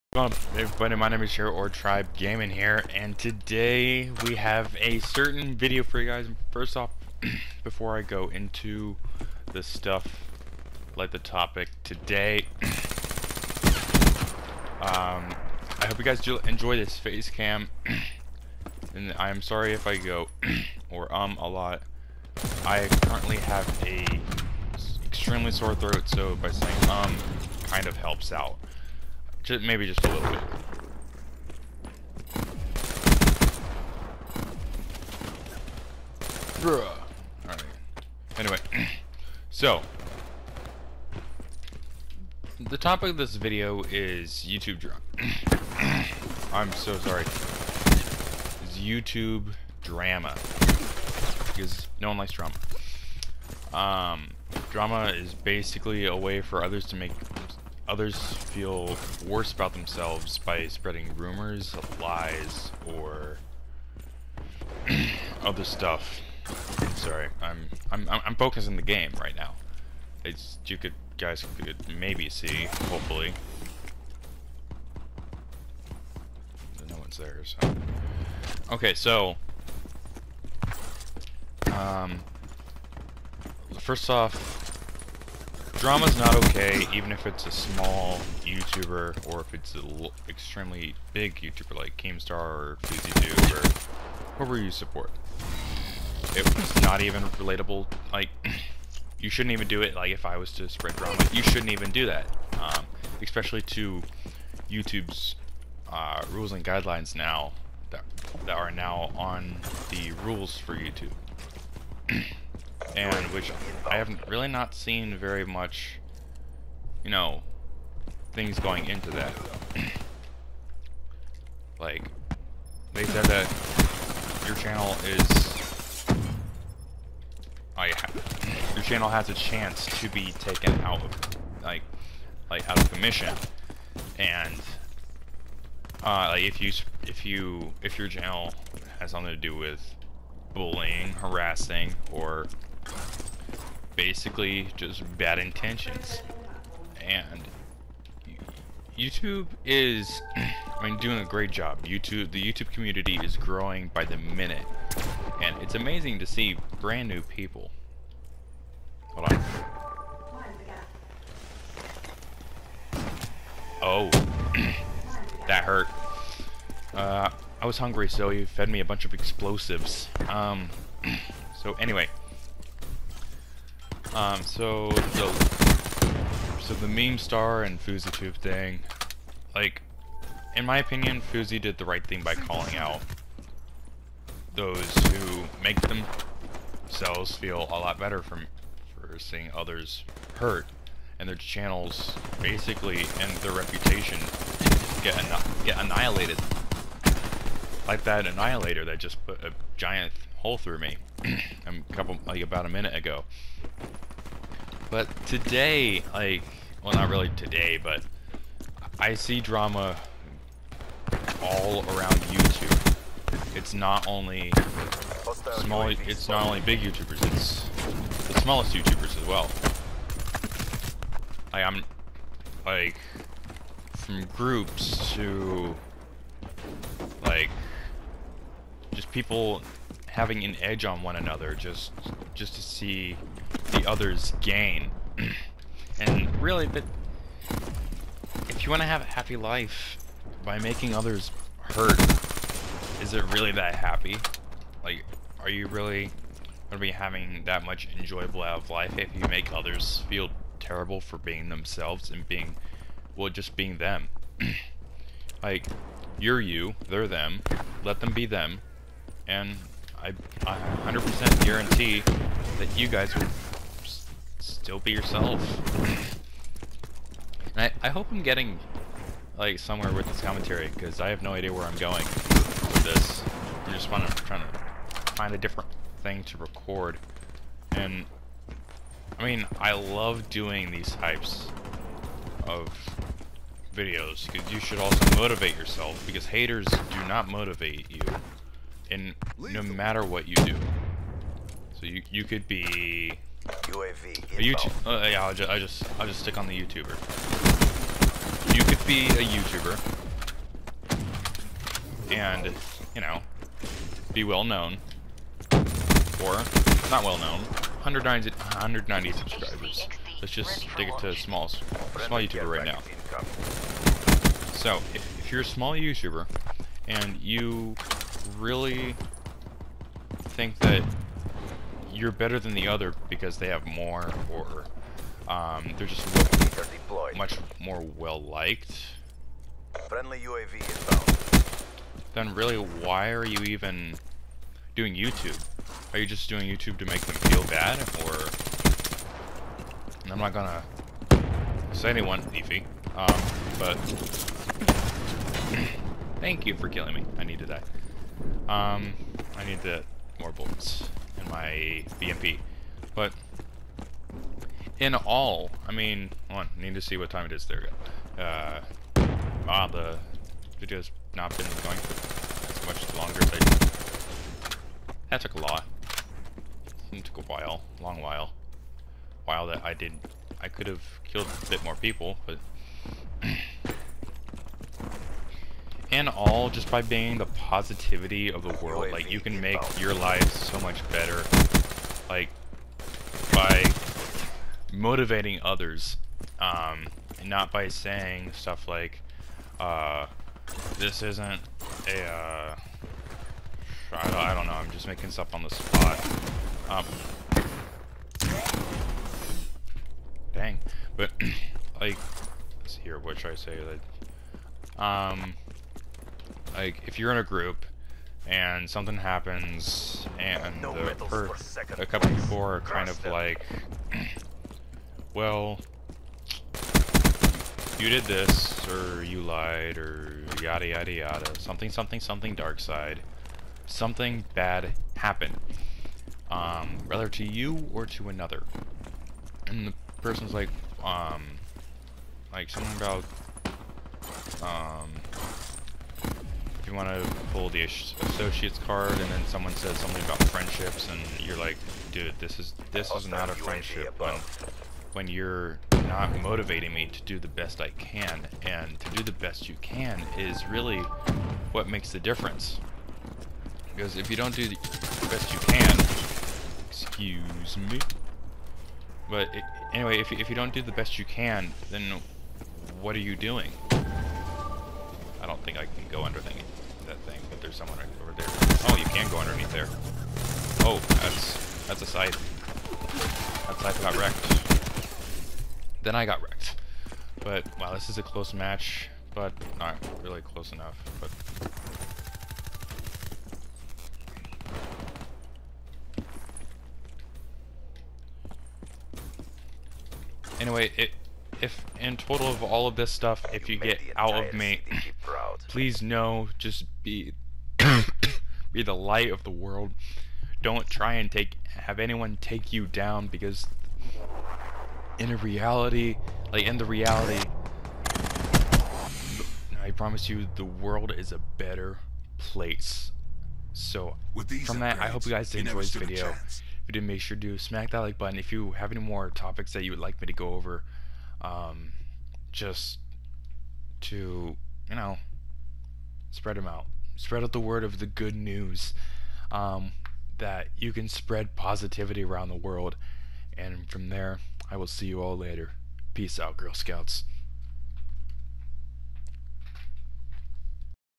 My Opinion on Youtube Drama
Drama is nothing that should be joked about, because its annoying and it serves absolutely no purpose to you or to others. If we as a YouTube Community can stop drama from happening,,, Don't you think we could all have a much better time doing what we love to do? Change a Life for the better of yourself and themselves. By you  Impacting one person it can impact many others!